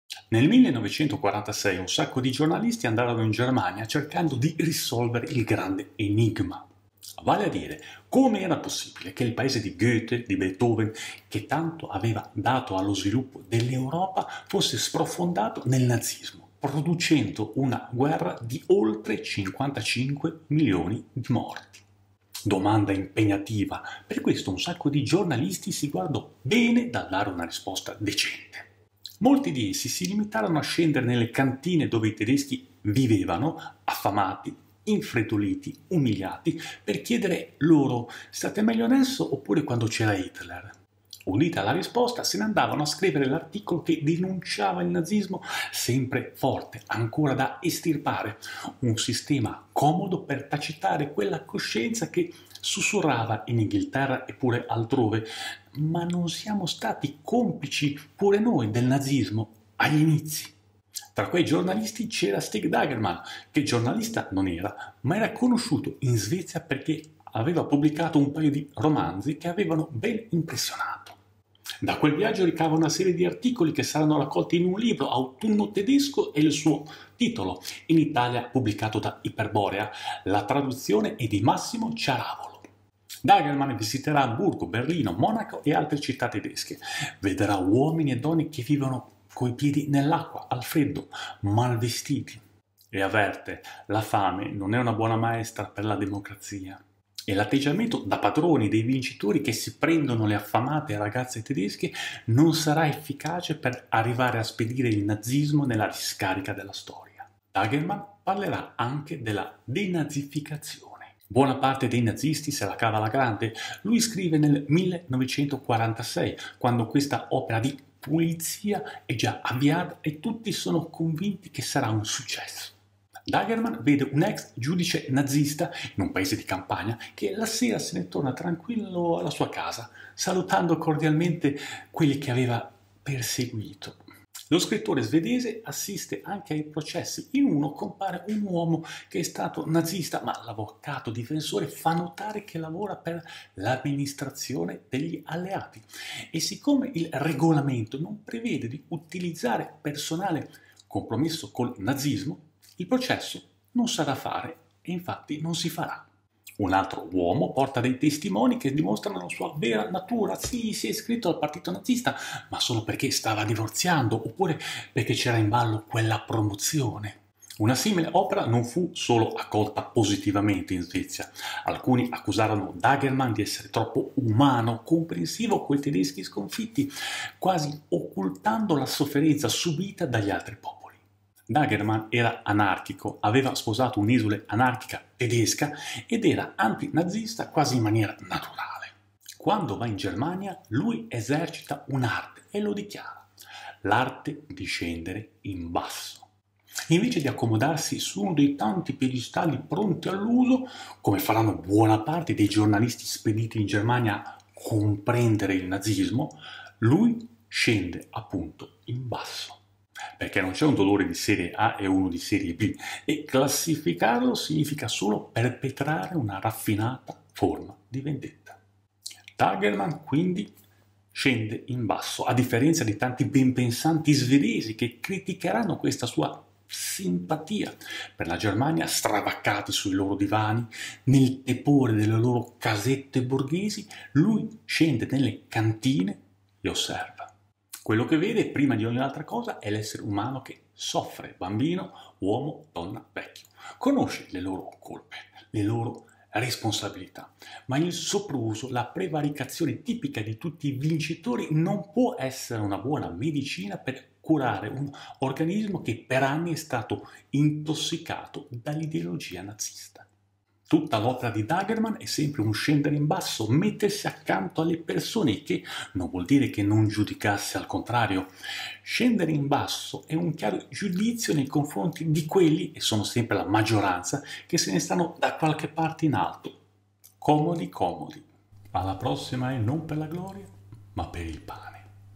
Autunno tedesco di Stig Dagerman - perché dovresti leggerlo?
Nel 1946, un sacco di giornalisti andarono in Germania cercando di risolvere il Grande Enigma. Vale a dire: come era possibile che il Paese di Goethe, di Beethoven, che tanto aveva dato allo sviluppo dell’Europa, fosse sprofondato nel nazismo, producendo una guerra di oltre 55 milioni di morti?

Chi è Marco Freccero? Esiste, oppure è un ologramma? SCOPRILO cliccando qui